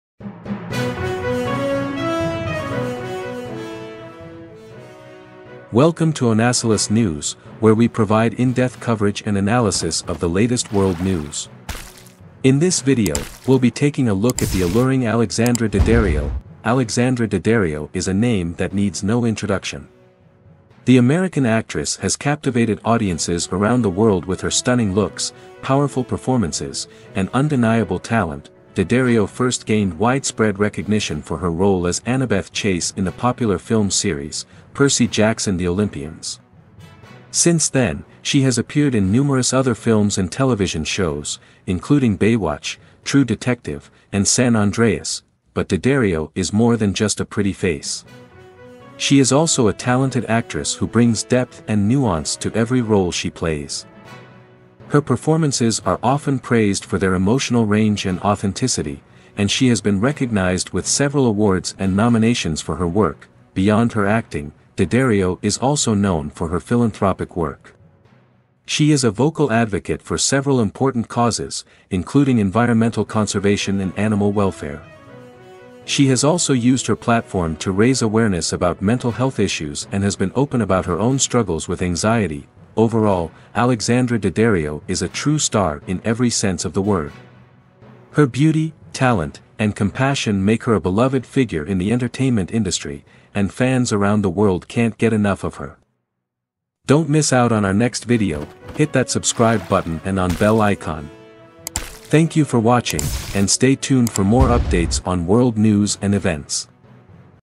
The Alluring Alexandra Daddario
Onasilus News brings you world news. Alexandra Daddario is a name that needs no introduction. The American actress has captivated audiences around the world with her stunning looks, powerful performances, and undeniable talent.